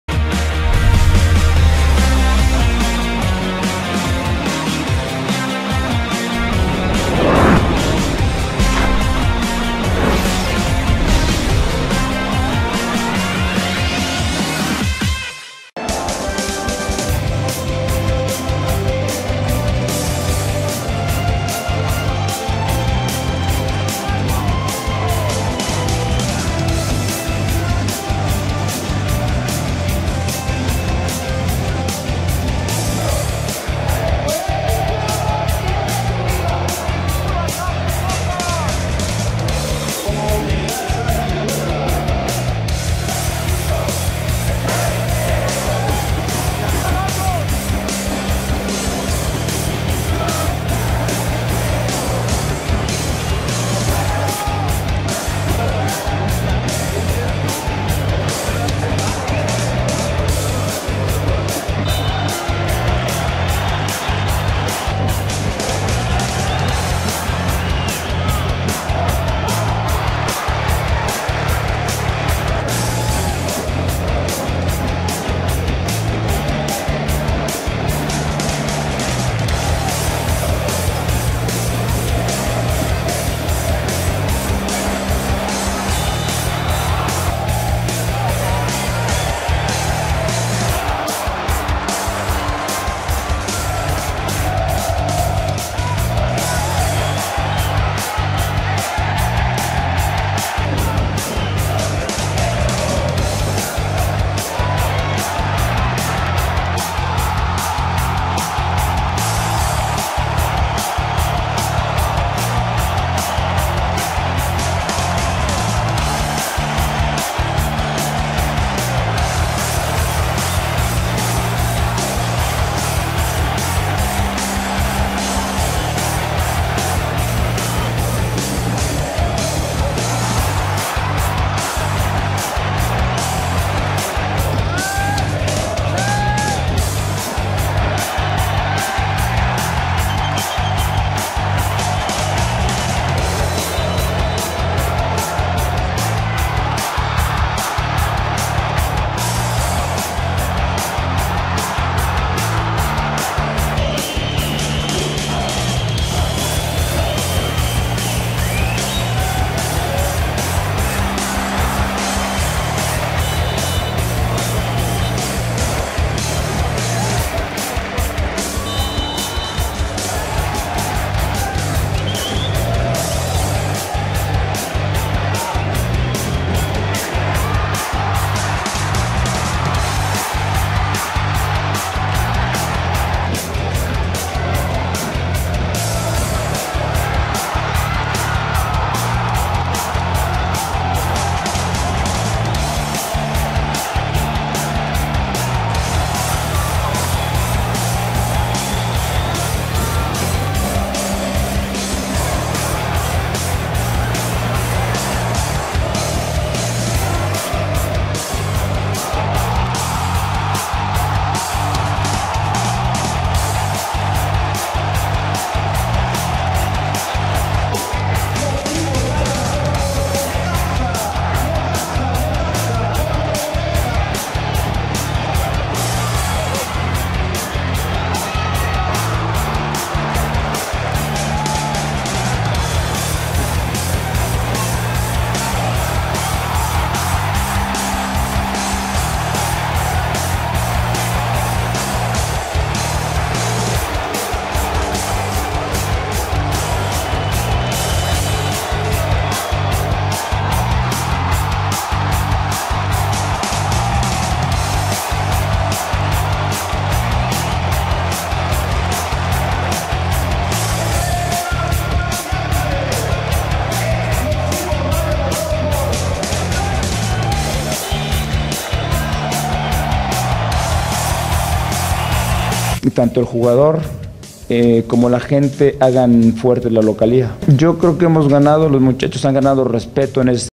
Necaxa Vs Puebla El Resumen.
Rayos del Necaxa recuperó su puesto en zona de Liguilla, luego de superar este día 3-1 a Puebla.

Aporte: Jorge Armando Lara.